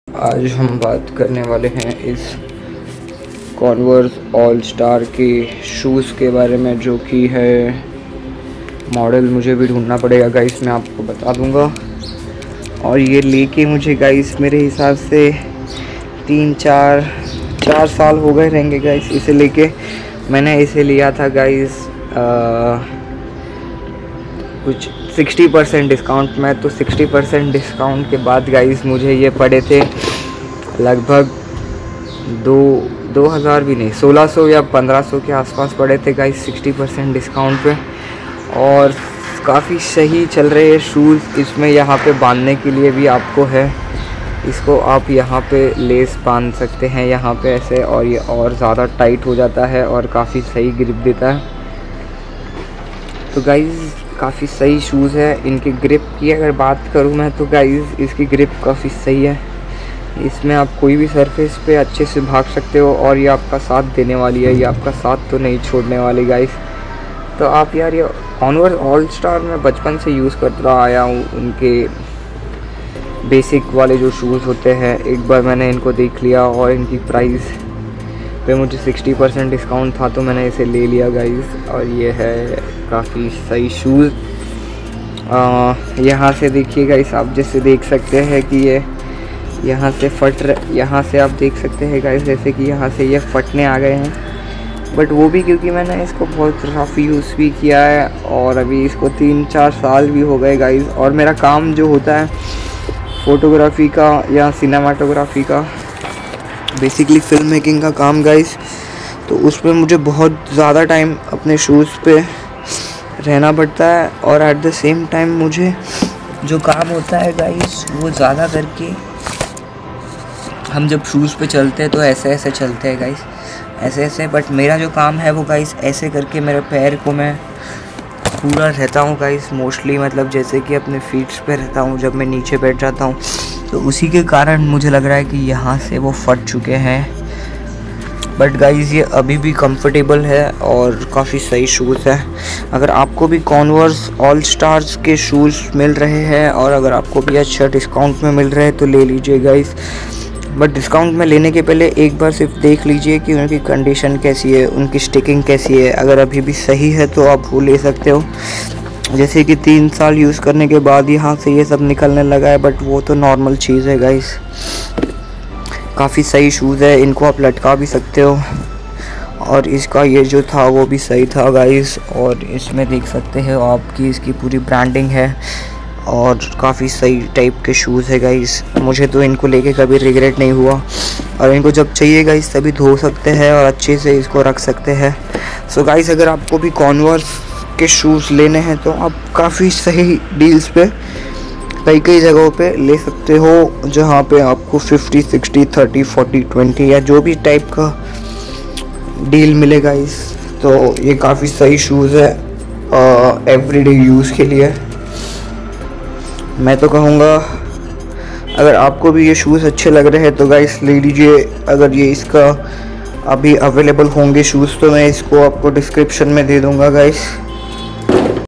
Converse All Star | Crazy Review | Bada Crazy
Review of my shoes that i took Some time ago.
I got it form Jeans Junction.